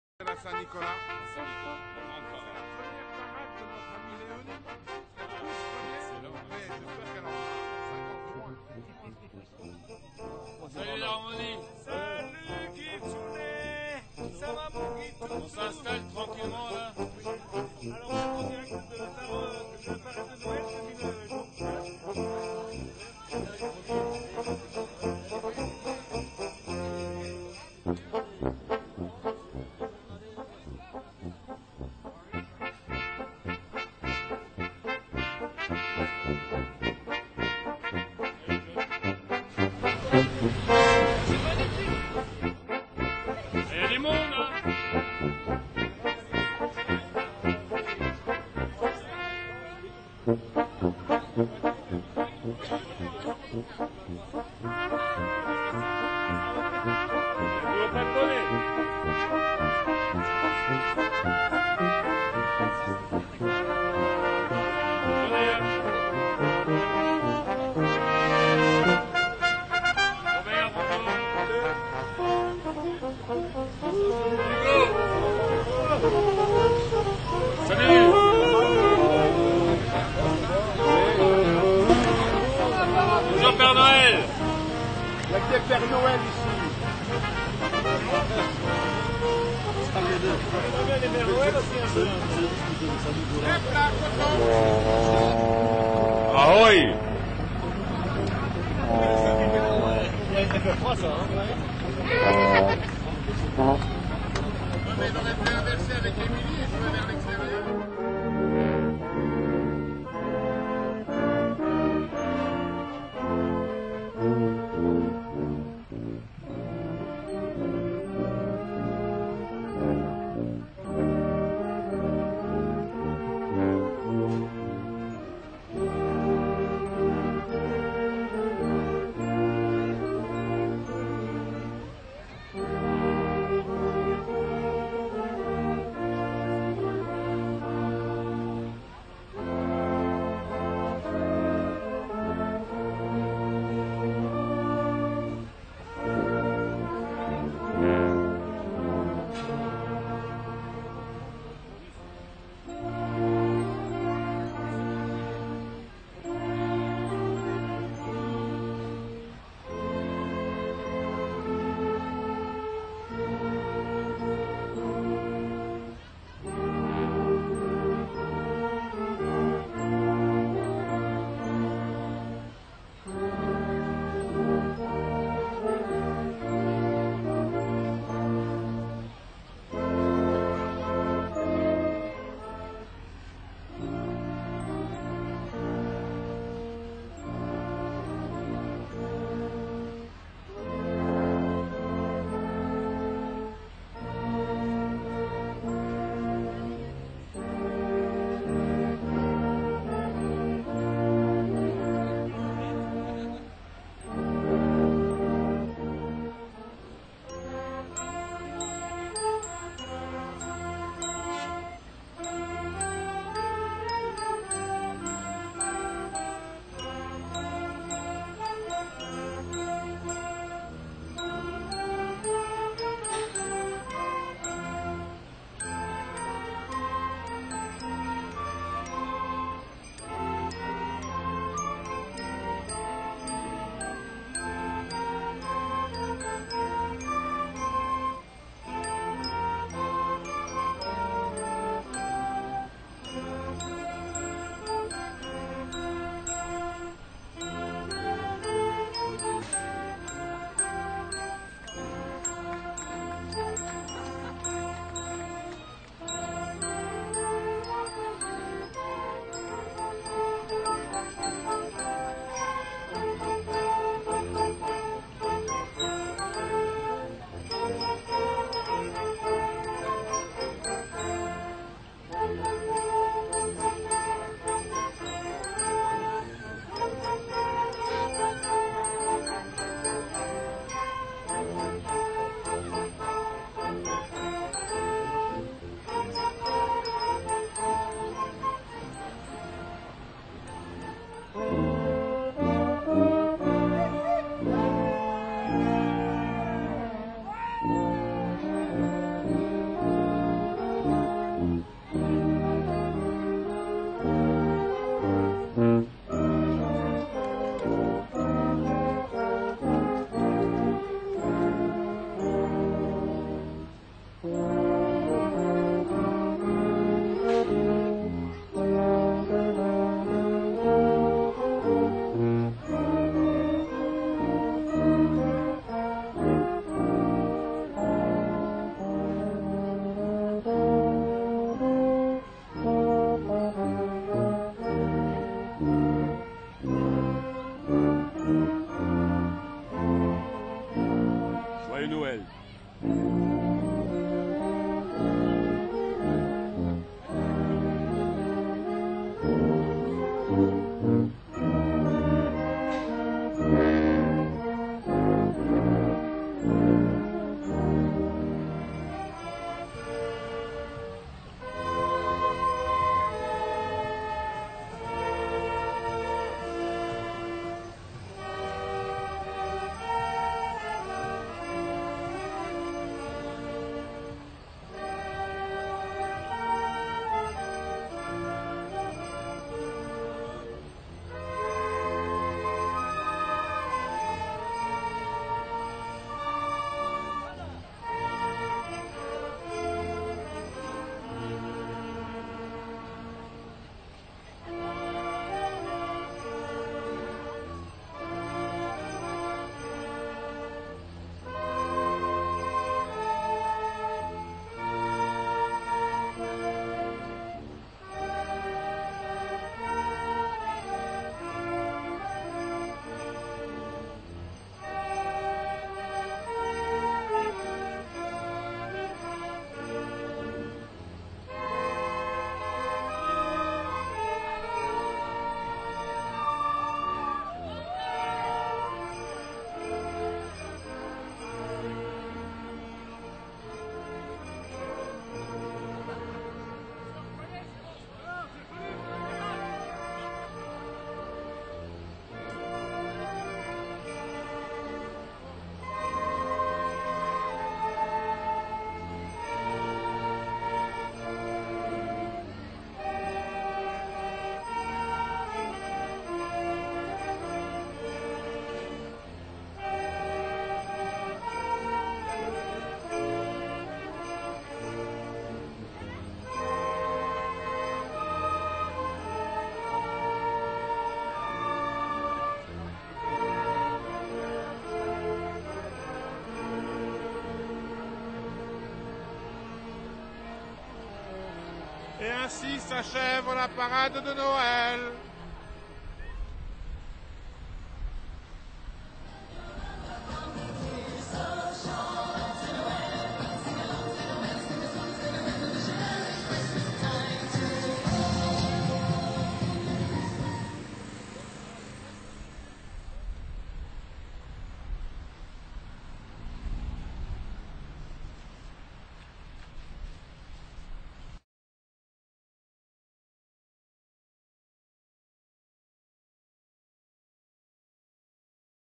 PARADE DE NOEL A SARREBOURG 2009
L'Harmonie Municipale de Sarrebourg participe régulièrement à la grande parade de Noël à Sarrebourg. Celle ci attire un public venu de toute la Lorraine et de l'Alsace proche. Grand évènement à Sarrebourg tous les ans une multitude d'autres participants font également la joie du public.
Vous découvrirez ce qui se passe sur notre char.